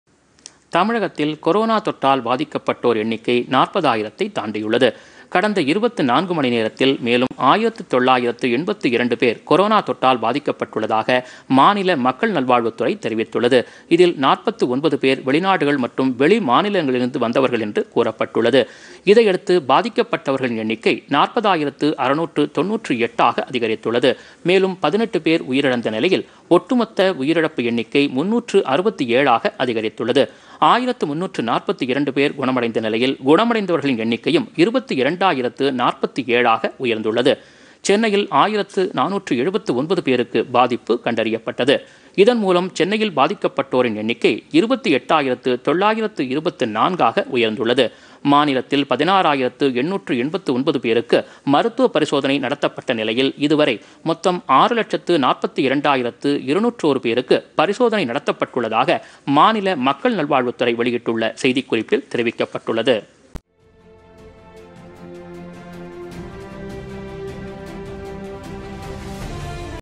தமிழகத்தில் பாதிப்பு 40 ஆயிரத்தை தாண்டியது
தமிழகத்தில் கொரோனா தொற்றால் பாதிக்கப்பட்டோர் எண்ணிக்கை 40 ஆயிரத்தை தாண்டியது- ஒரேநாளில்  ஆயிரத்து 982 பேருக்கு தொற்று
- மாநில மக்கள் நல்வாழ்வுத் துறை